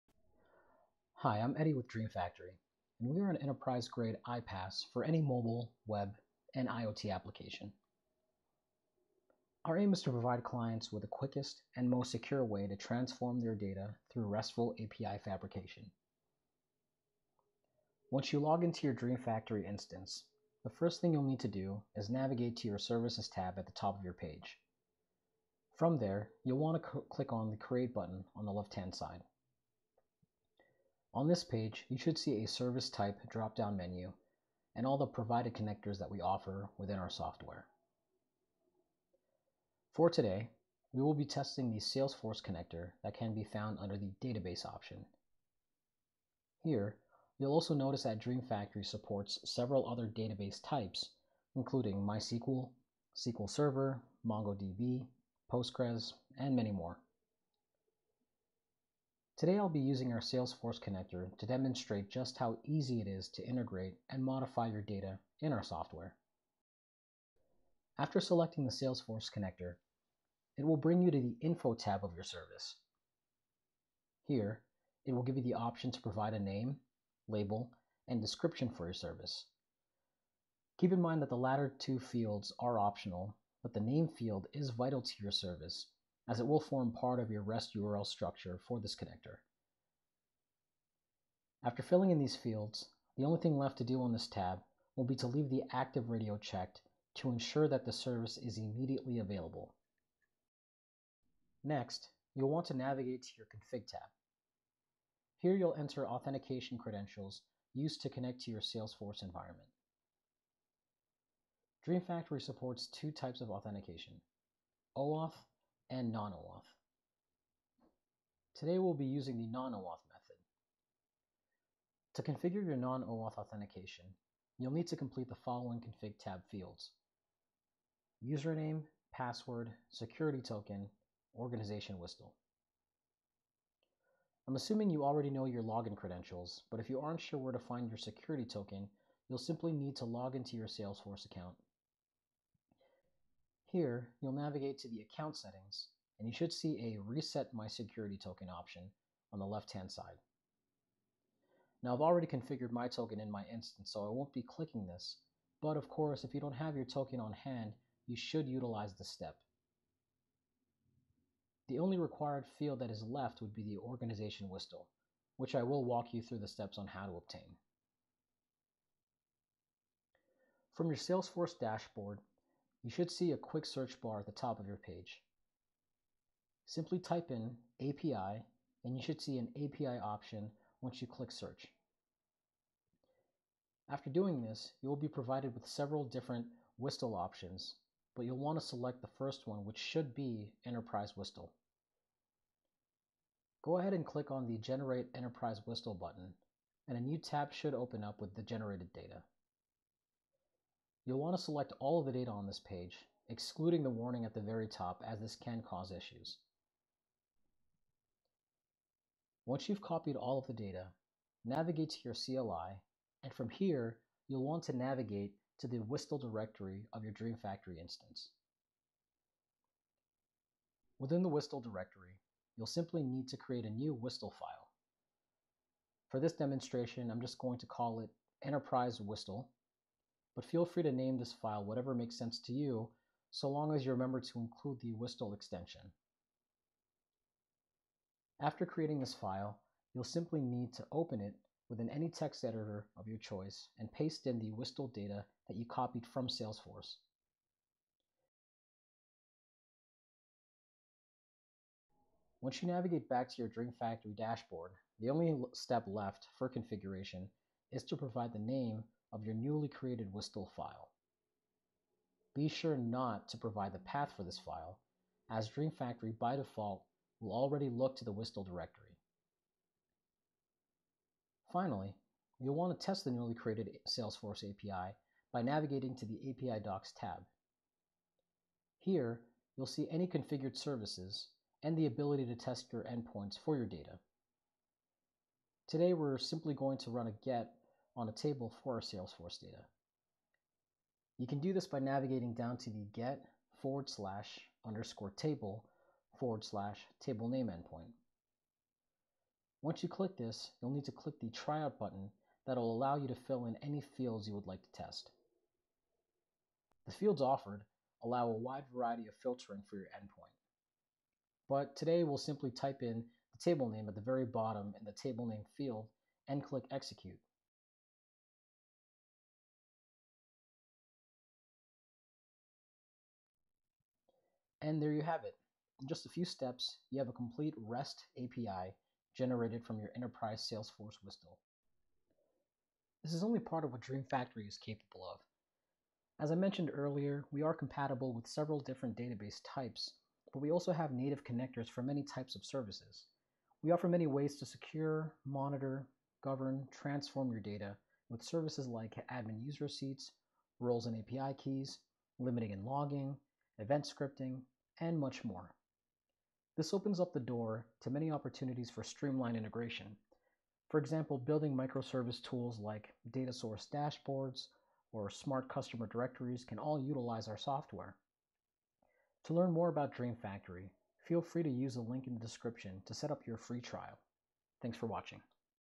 Instant Salesforce REST API Creation
DreamFactory instant Salesforce REST API Creation via a low code API management platform.

Salesforce is a prolific CRM platform that comes with significant complexities. There are multiple sources of data constantly travelling in and out of the system, being accessed by multiple users. The value of this data cannot be understated and there is an inherent requirement that this data is analysed and manipulated by any number of systems.

The Salesforce CRM is crucial to business operations for millions of organizations. Data often needs to be moved between Salesforce and other technologies such as SQL or NoSQL databases. Using DreamFactory's powerful integration platform you can easily create sophisticated workflows for moving data between Salesforce and any number of destinations.